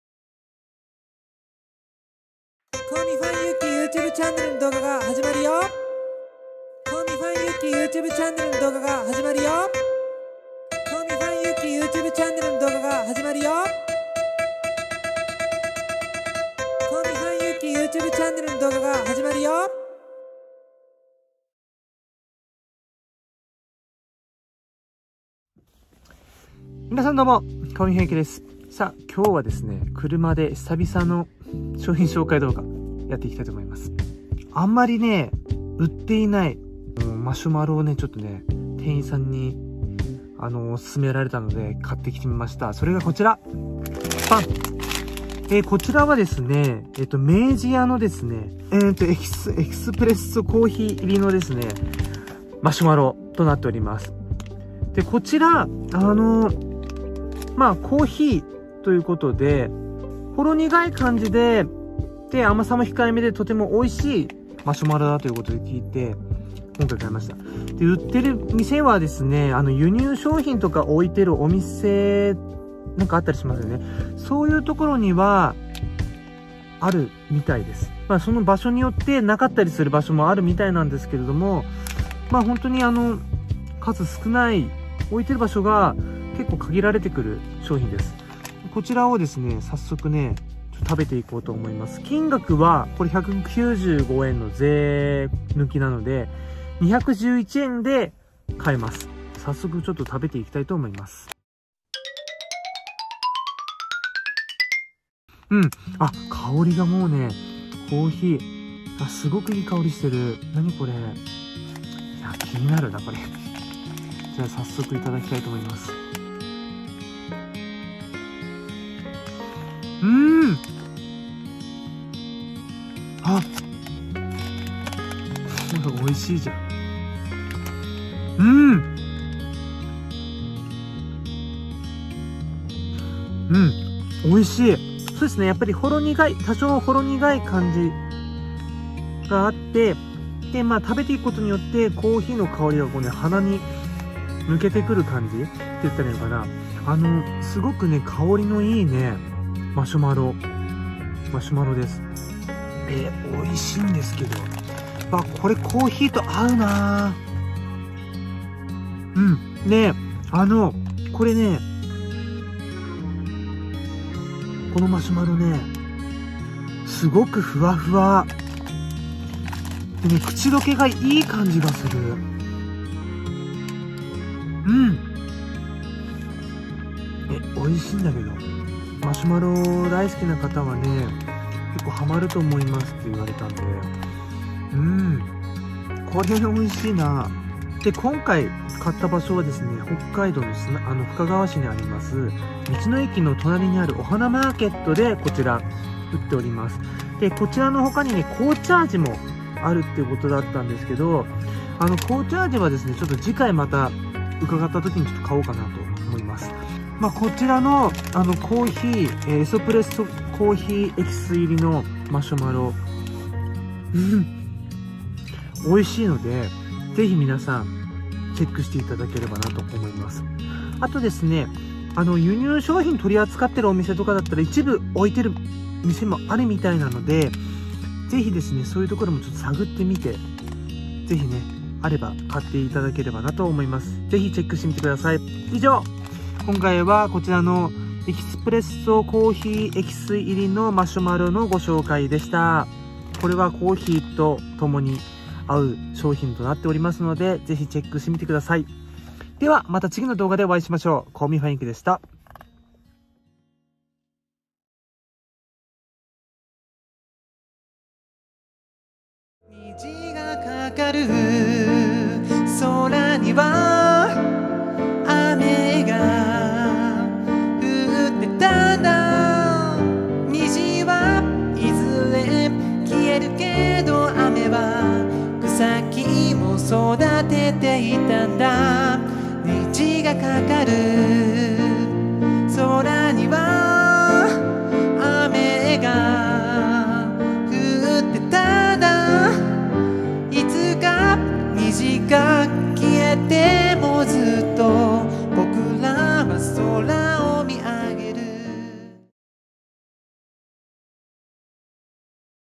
4K【 激うま お菓子 】明治屋 コーヒーマシュマロ エスプレッソコーヒーエキス入り が美味すぎた!。
北海道深川市 OHANAmarket で購入しました。